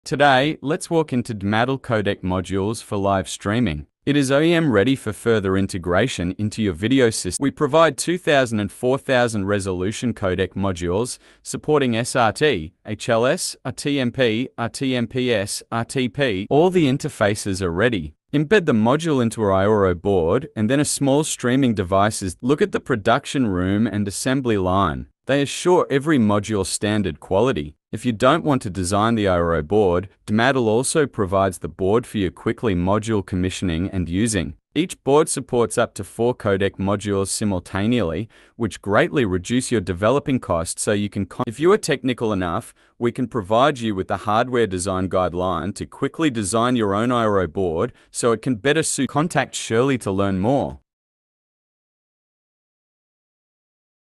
DDMALL 2K/4K HDMI codec module for live streaming codec broadcast livestreaming video srt live
the OEM-ready codec module is developed by DDMALL for customer further development and integration. it is mini andl lightweight with built-in H.265 H.264 srt, hls, rtmp, rtmps, rtsp, rtp, tcp, udp, multicast, unicast. Customer can buy DDMALL I/O board for these modules functioning, or they can design own I/O board for better integration into the video systems. DDMALL will provide the I/O board hardware design guidelines. With this module, you can do your own live streaming systems without developing from the beginning, saving your energy and reducing your cost at most. You will also focus yourself on other critical tasks.